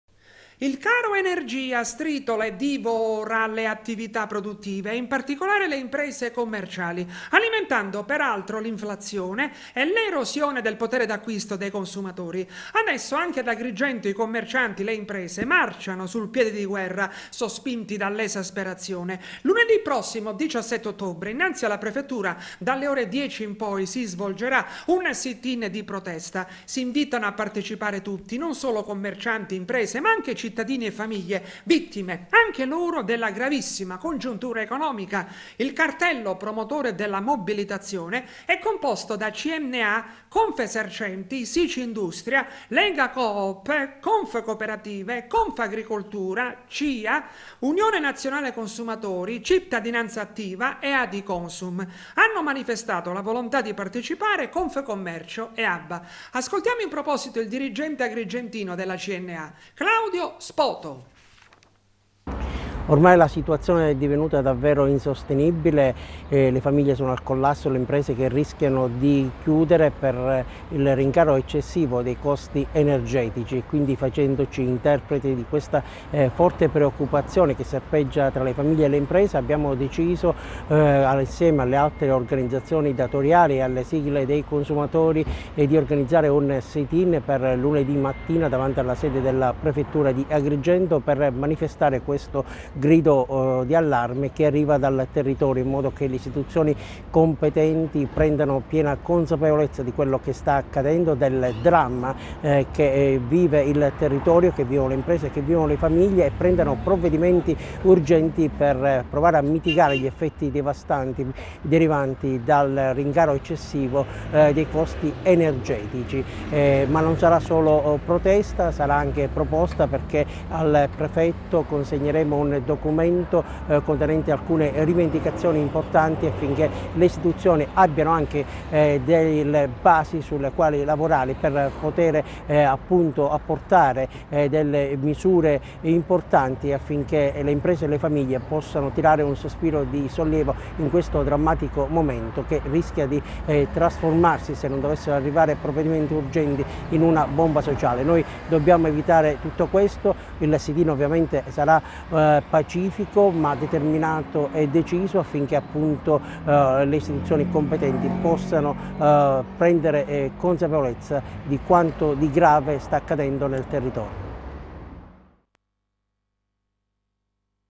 Teleacras - Caro energia, Cna lancia protesta ad Agrigento
Il caro energia stritola e divora le attività produttive e, in particolare, le imprese commerciali, alimentando peraltro l’inflazione e l’erosione del potere d’acquisto dei consumatori. Adesso anche ad Agrigento i commercianti marciano sul piede di guerra, sospinti dall’esasperazione. Lunedì prossimo, 17 ottobre, innanzi alla Prefettura, dalle ore 10 in poi si svolgerà un sit – in di protesta. Si invitano a partecipare tutti, non solo commercianti ma anche cittadini e famiglie, vittime anche loro della gravissima congiuntura economica. Il cartello, promotore della mobilitazione è composto da Cna, Confesercenti, Sicindustria, Legacoop, Confcooperative, Confagricoltura, Cia, Unione Nazionale Consumatori, Cittadinanza Attiva e Adiconsum.  Hanno manifestato la volontà di partecipare Confcommercio e Abba. Ascoltiamo in proposito il dirigente agrigentino della Cna, Claudio Spoto…